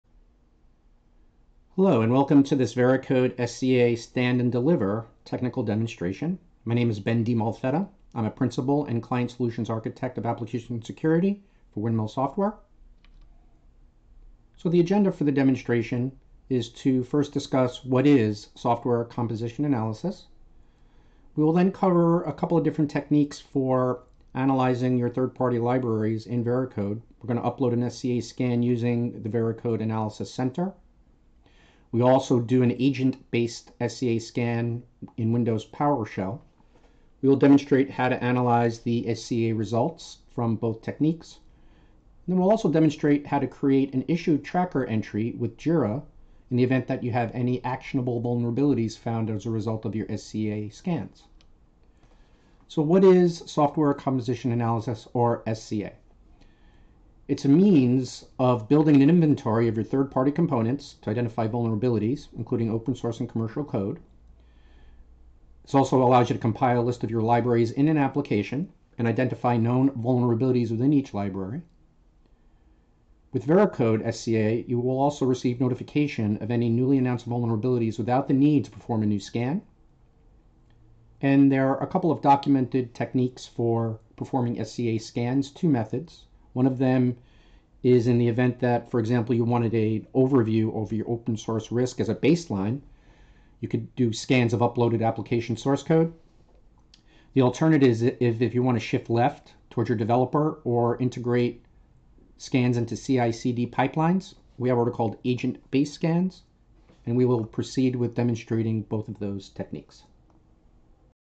Veracode Software Composition Analysis (SCA) Scan Demo Overview
In this video series, we will summarize what is Software Composition Analysis (SCA) and how to utilize Visual Studio, the native Veracode platform as well as PowerShell to invoke a Software Composition Analysis (SCA) scan.  We will also analyze the scan results within Veracode and demonstrate how to use the Veracode/JIRA integration to create a ticket that will track resolution of an actionable SCA finding.